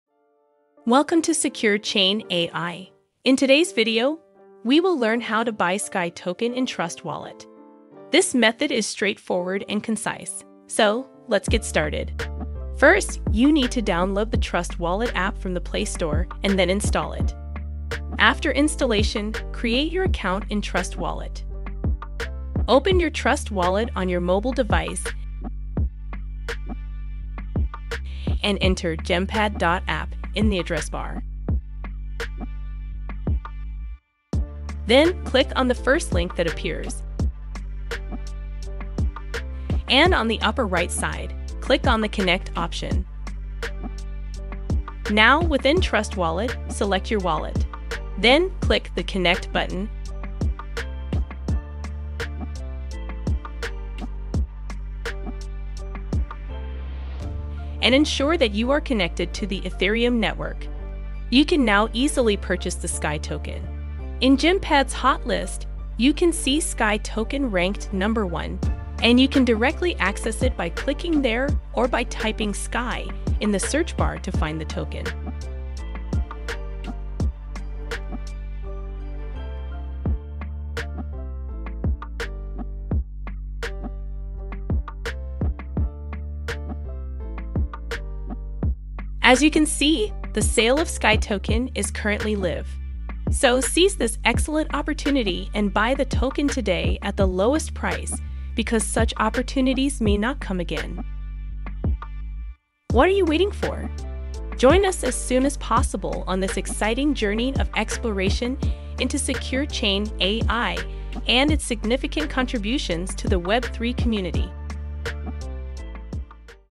How To Buy SCAI Token In GemPad With Trust Wallet!
Welcome to SecureChain AI.

In today's video, we will learn how to buy SCAI token in trust wallet. This method is straightforward and concise. So, Let's get started.

First, you need to download the Trust Wallet app from the Play Store and then install it. After installation, create your account in Trust Wallet.

Open your Trust Wallet on your mobile device and enter "Gempad.app" in the address bar. Then, click on the first link that appears, and on the upper right side, click on the "Connect" option.

Now, within Trust Wallet, select your wallet. Then, click the "Connect" button and ensure that you are connected to the Etherium Network.

You can now easily purchase the SCAI Token. In Gempad's hot list, you can see SCAI Token ranked number 1, and you can directly access it by clicking there or by typing "SCAI" in the search bar to find the token.

As you can see, the sale of SCAI Token is currently live. So, seize this excellent opportunity and buy the token today at the lowest price because such opportunities may not come again.

What are you waiting for? Join us as soon as possible on this exciting journey of exploration into SecureChainAI and its significant contributions to the Web3 community.